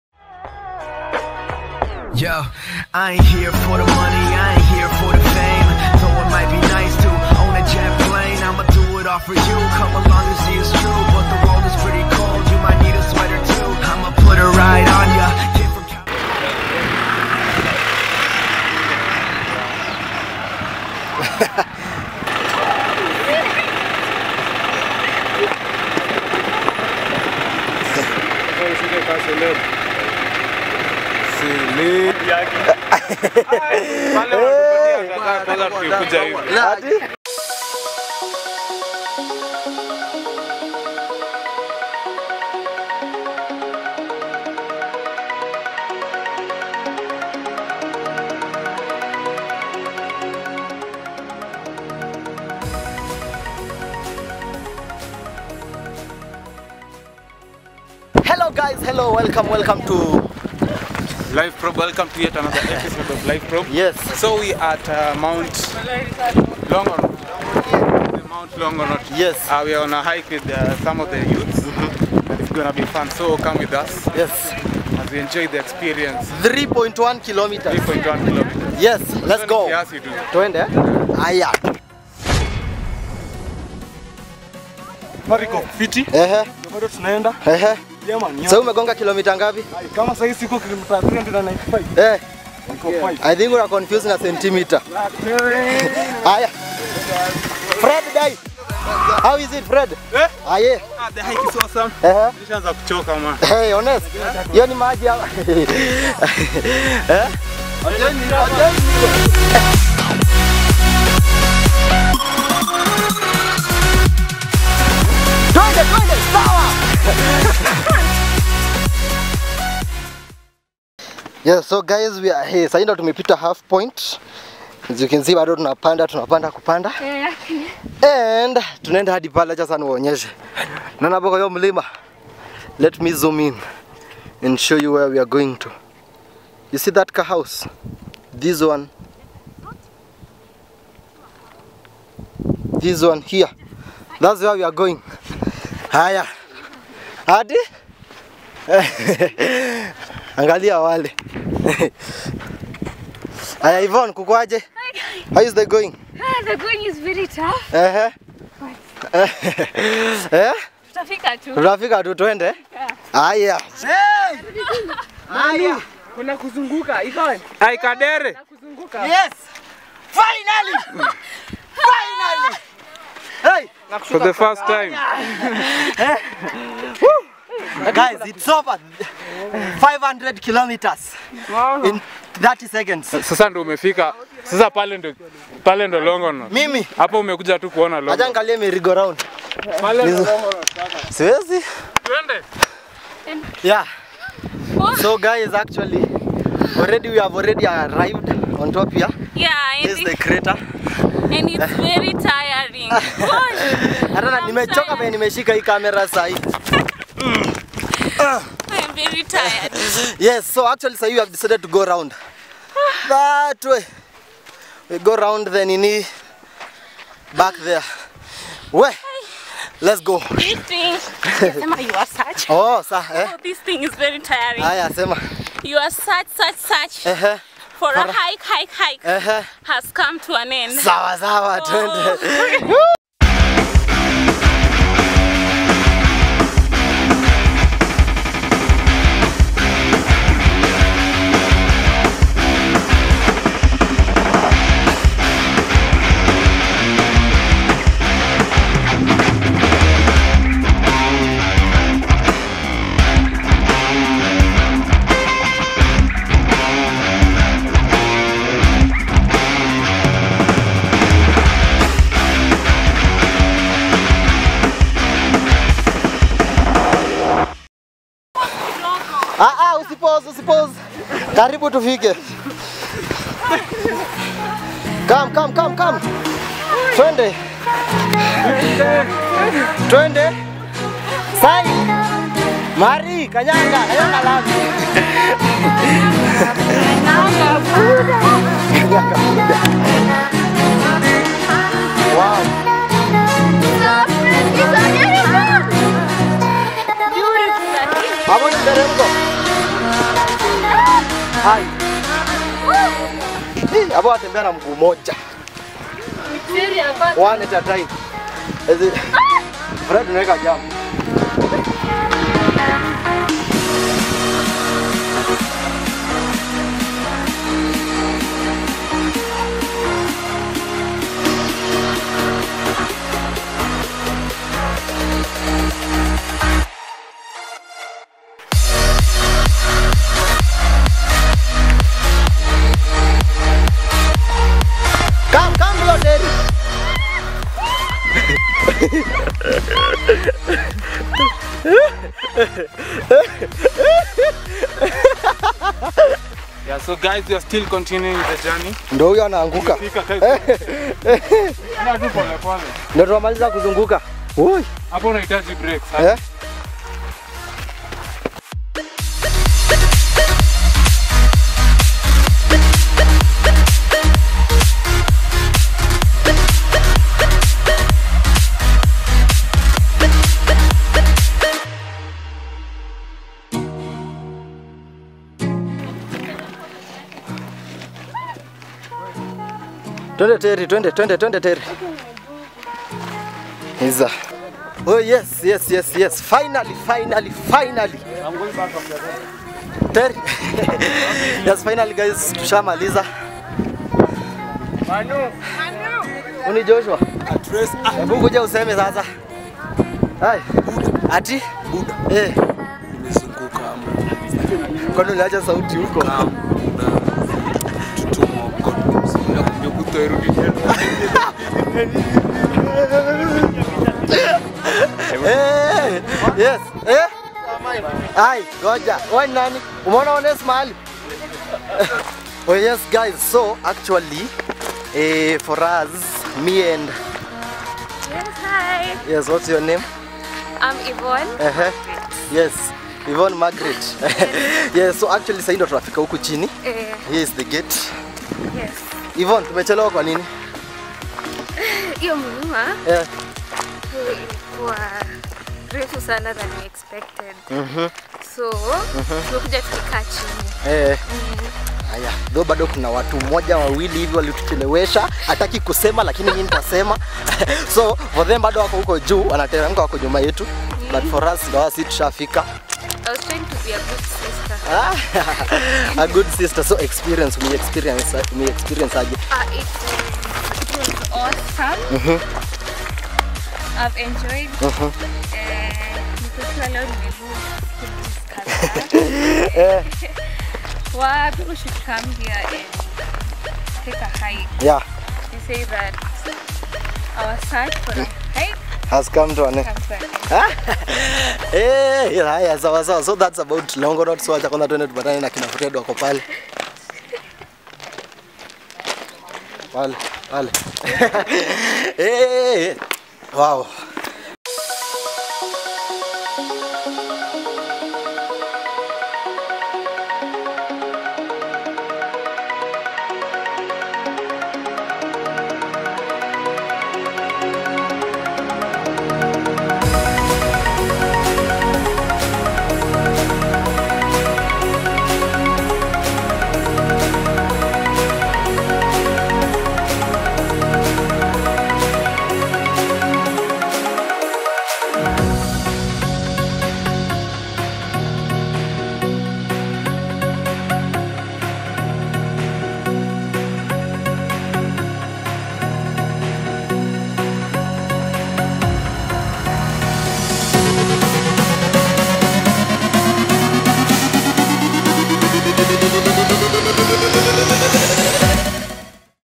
HIKING MT LONGONOT, AND GOING ROUND THE CRATER
We went hiking to Mt longonot and the experience is great. 3.1 KM and 7.2KM round the crater.